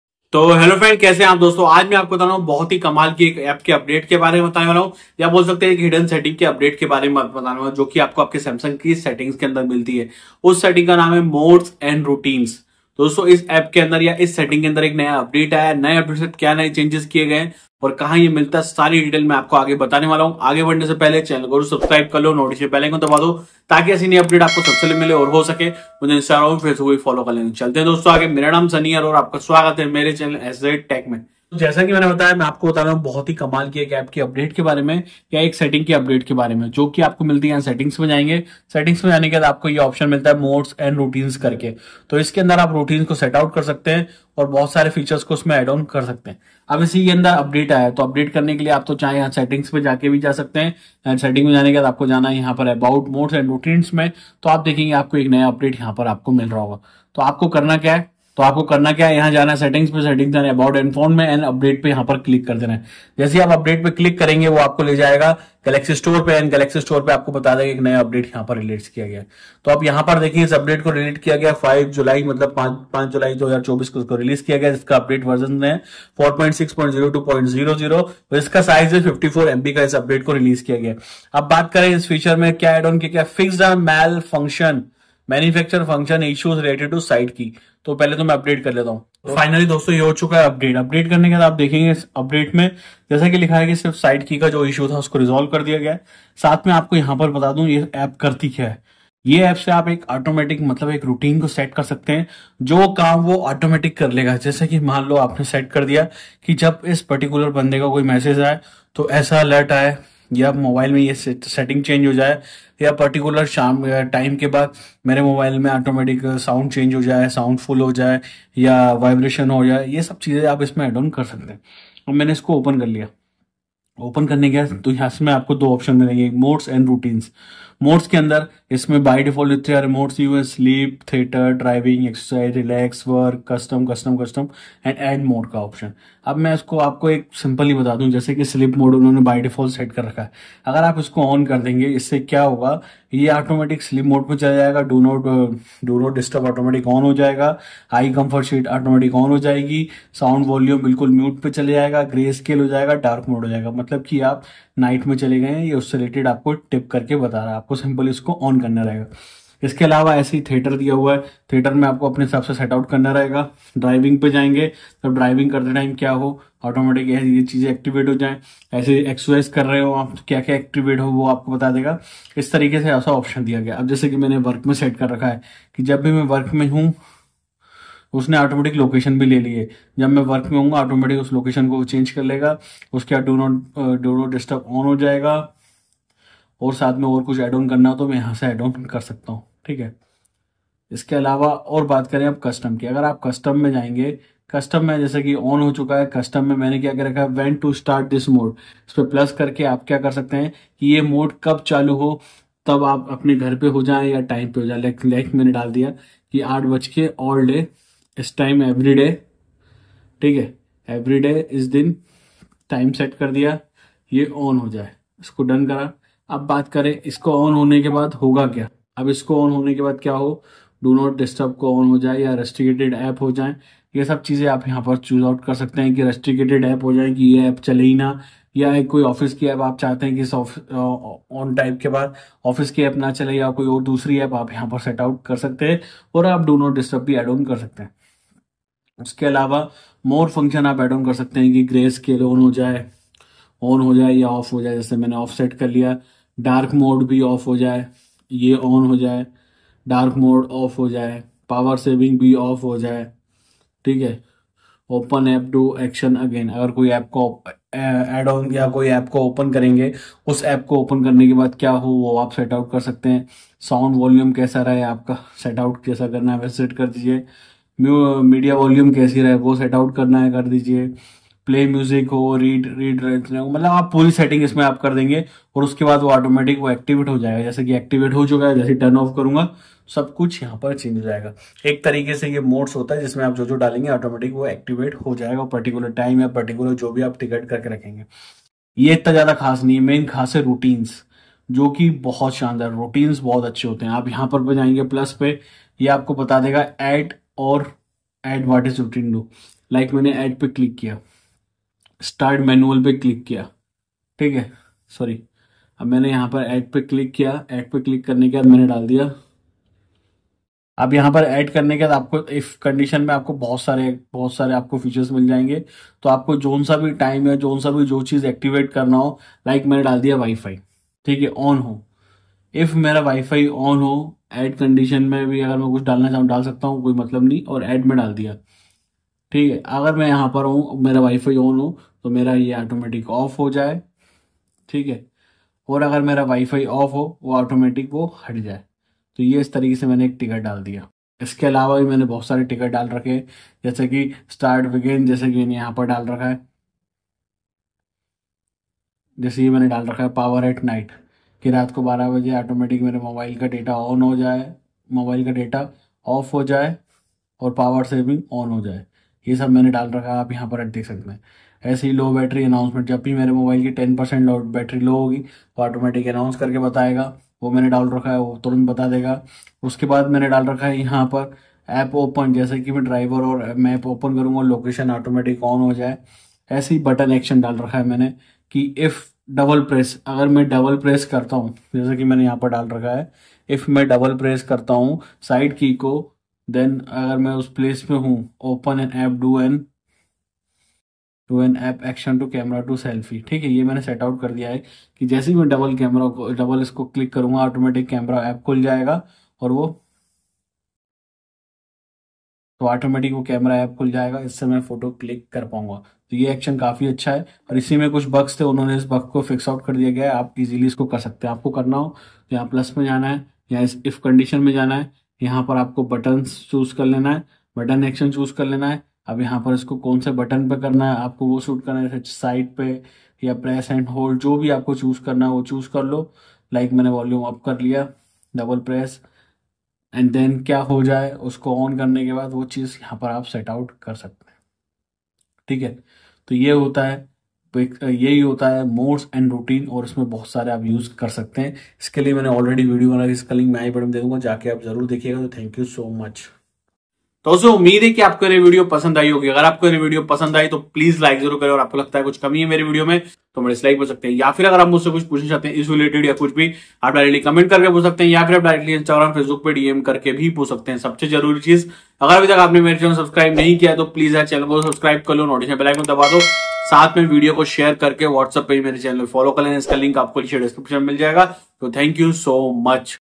Samsung Modes & Routines Get a Big Upgrade 2024
in this video i describe about samsung modes & rountines new update 2024